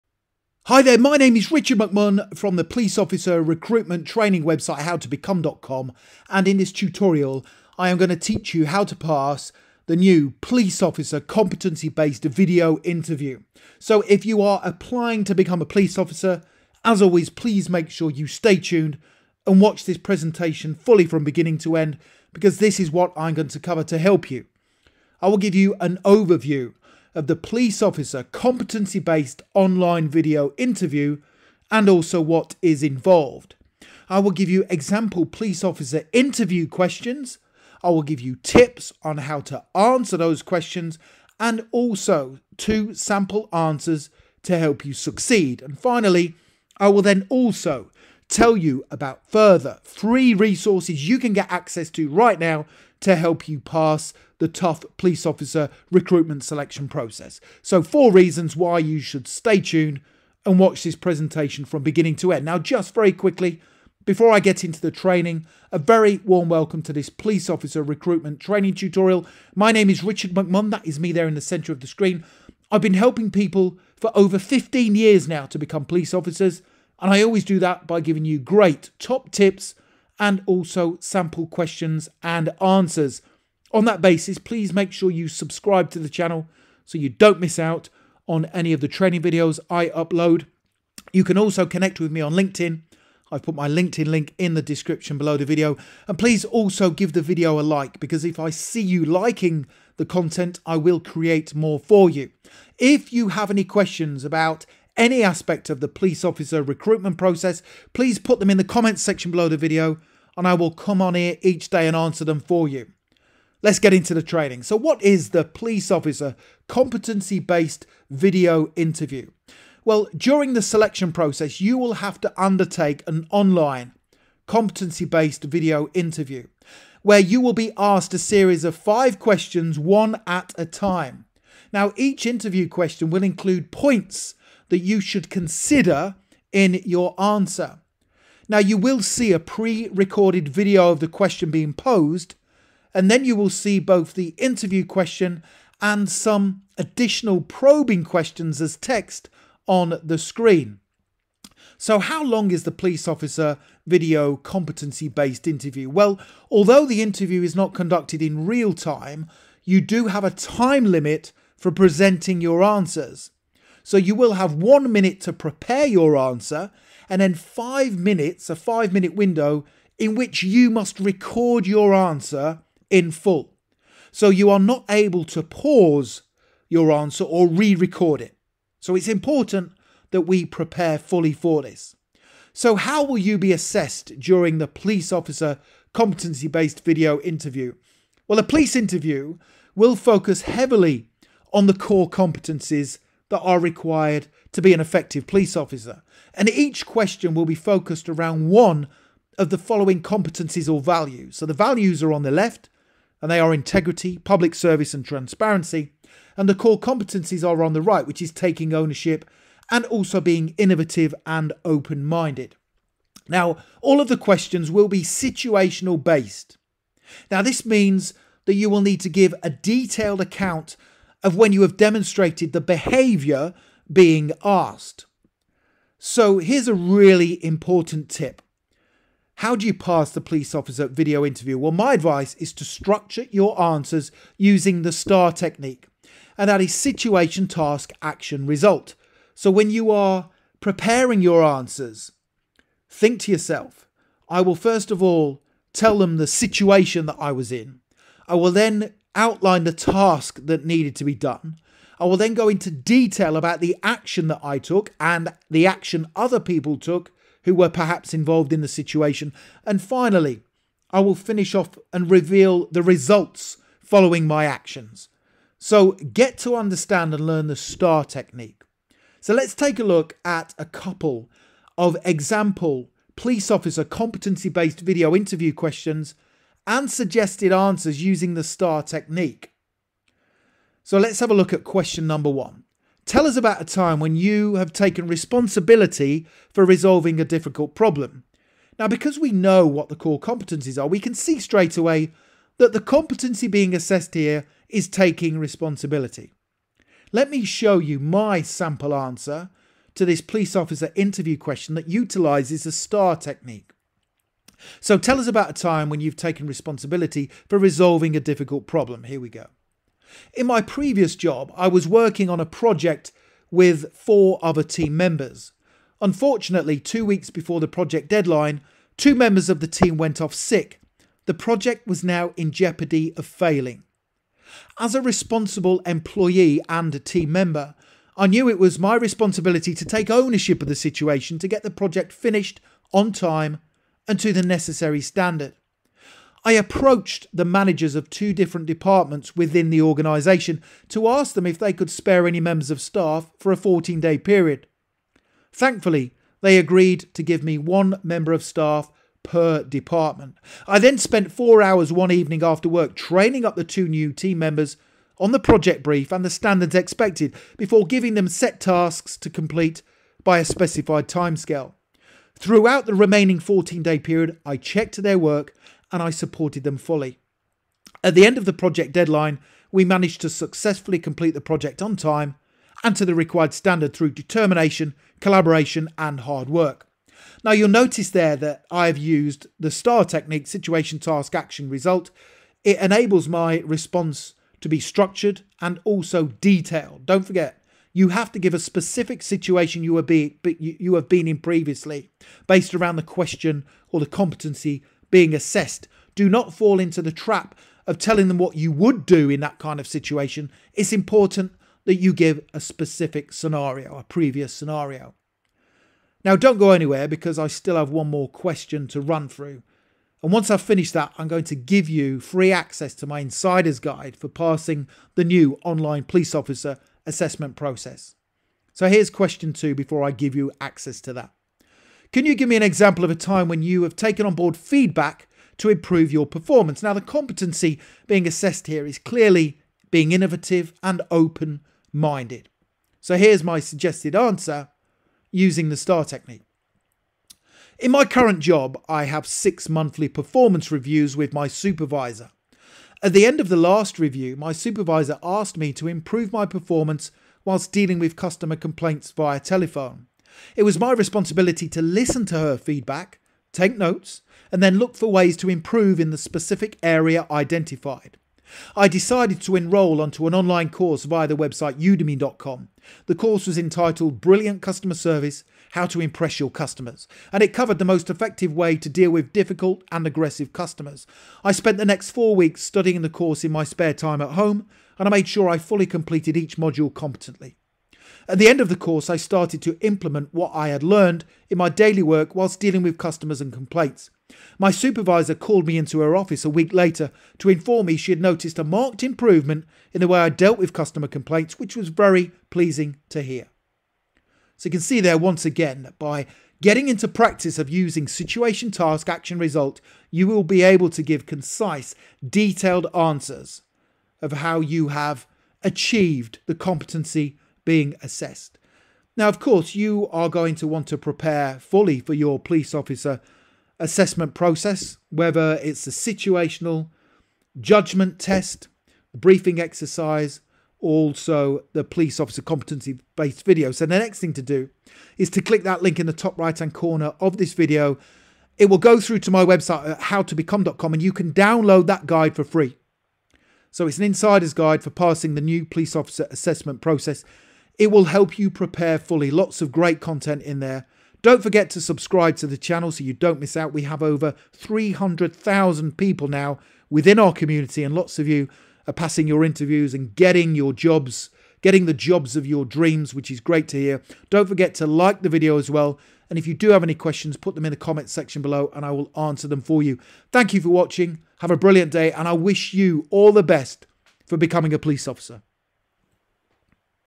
Police Online Assessment Process: (Stage 2) Competency-Based VIDEO INTERVIEW Questions & Answers!
Police Online Assessment Process: Police Officer Competency-Based Video Interview by Richard McMunn
In this police recruitment training video, Richard McMunn teaches what is involved in the new Police Officer and Police Constable Competency-Based Online Video interview assessment introduced by the college of policing including:
1. An overview of the Police Officer Online Video Interview, the new online assessment process, and what is involved;
2. Example Police Officer Interview questions;
3. TIPS on how to ANSWER the Police Video Interview questions;
4. Further FREE Police Recruitment resources to help you.

WHAT IS THE POLICE OFFICER COMPETENCY-BASED VIDEO INTERVIEW?
- During the competency-based interview, you will be asked a series of five questions, one at a time.
- Each question will include points that you should consider in your answer.
- You will see a pre-recorded video of the question being posed, and then see both the interview question and some additional probing questions as text on the screen.

HOW LONG IS THE POLICE OFFICER VIDEO COMPETENCY-BASED INTERVIEW?
- Although the interview is not conducted in real-time, you do have a time-limit for presenting your answers.
- You will have one minute to prepare your answer, followed by a 5-minute window in which you must record your answer, in full.
- You will not be able to pause or re-record your answer.

HOW WILL I BE ASSESSED DURING THE POLICE OFFICER VIDEO INTERVIEW?
All of the questions will be situational based. This means that you will need to give a detailed account of when you have demonstrated the behaviour being asked for. Use the STAR technique to structure your answers!

POLICE OFFICER COMPETENCIES AND VALUES
Integrity
Public Service
Innovative and Open-mindedness
Transparency
Taking Ownership

EXAMPLE POLICE OFFICER ONLINE VIDEO INTERVIEW QUESTIONS
Q. Tell us about a time when you have taken responsibility for resolving a difficult problem.
Q. Can you give me an example of a time when you have taken on board feedback to improve your performance?